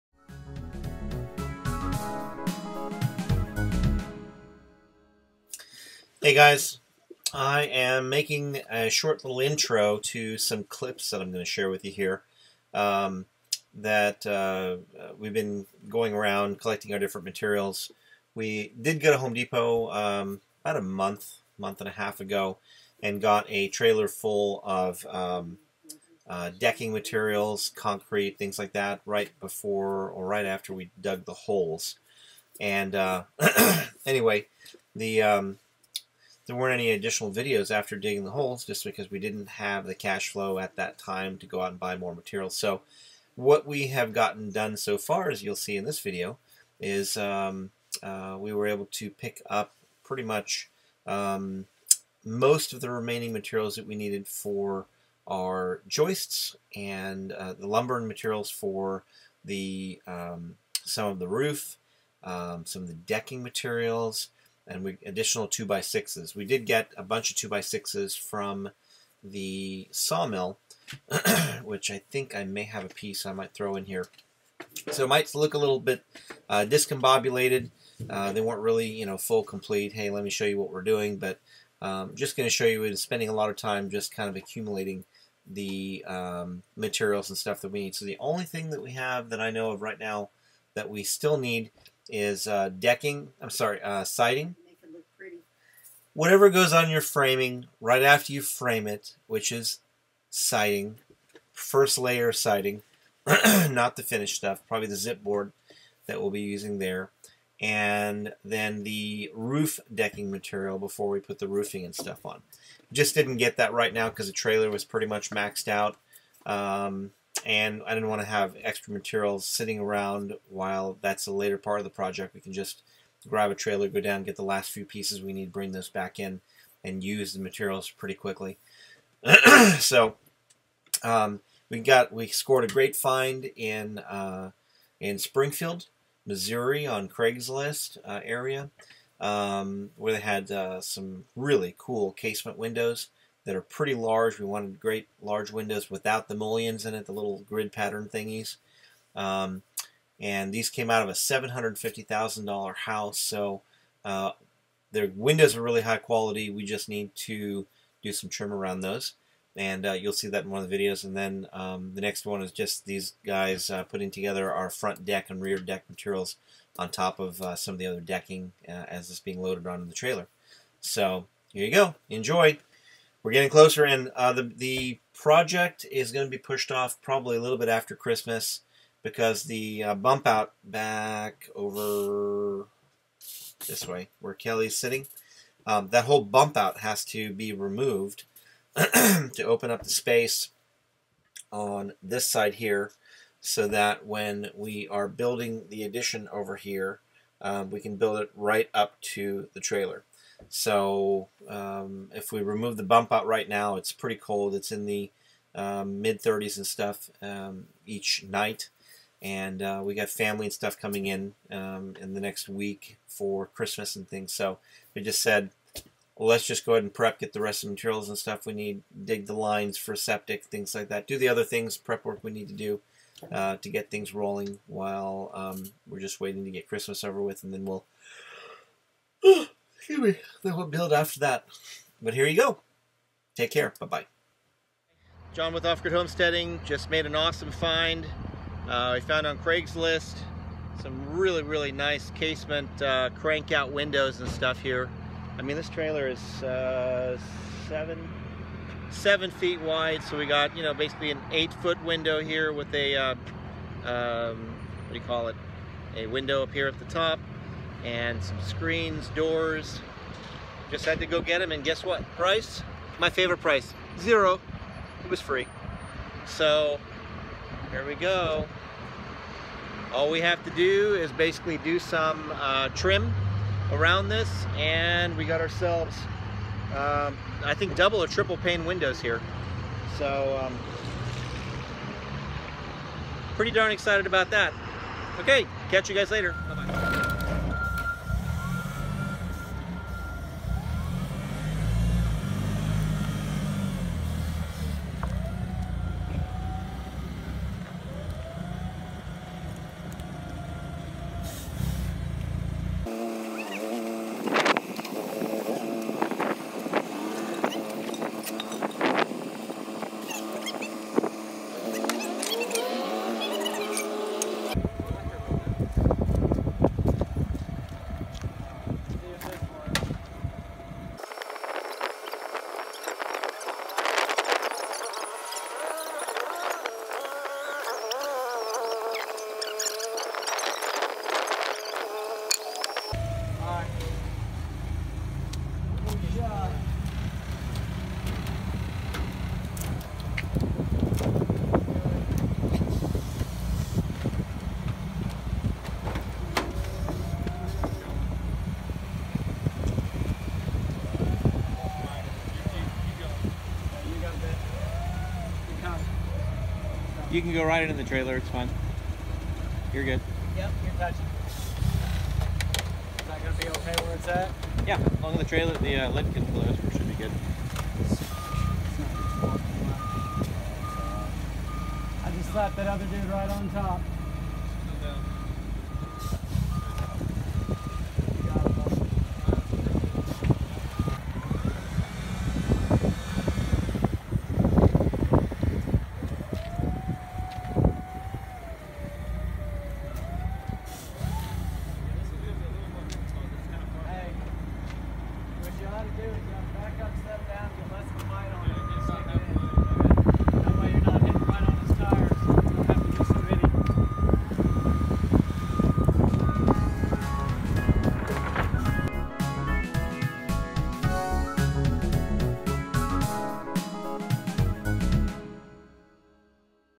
OGH - How to Get $1000s of Free Windows Off Grid
DS

John & Kelly have been investing a lot of time searching for windows, and lumber to have most or all of the materials required to build the addition.  The fact that the bump out of the trailer has to be completely removed before starting the addition, is the holdup at this point at night temperatures are in the low 30's, and highs have only been getting in to the 40's to 60's.  With Christmas coming up soon, we wanted to hold off on tearing out the bump out as it would dramatically lower the seating / walking space while we have 5 family members coming over for Christmas.

Want to get inside our heads and understand how we started thinking about homesteading?